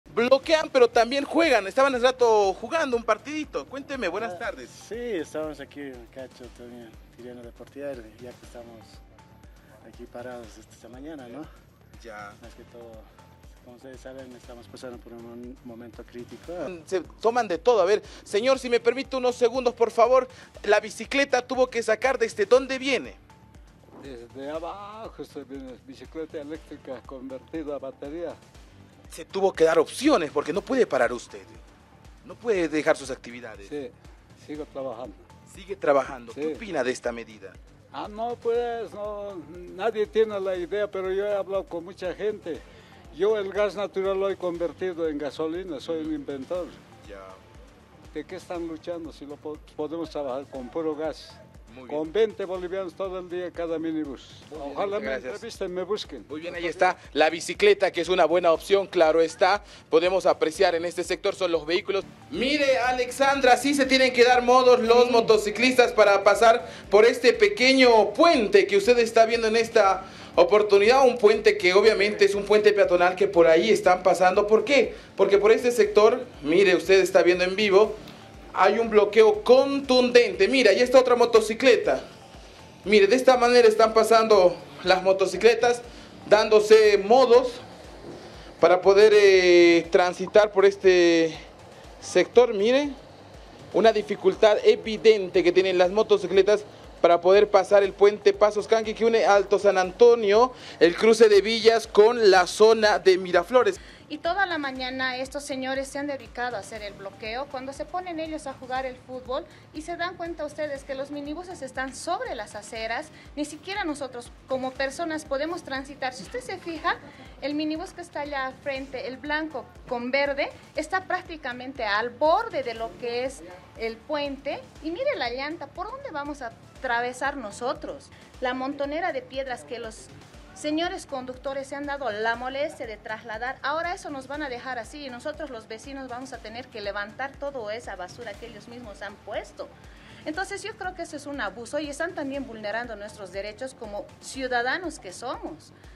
Mientras algunos bloqueadores jugaban fútbol, ciudadanos encontraron formas de movilizarse
Mientras algunos bloqueadores jugaban fútbol, ciudadanos encontraron formas de movilizarse durante la jornada de bloqueos.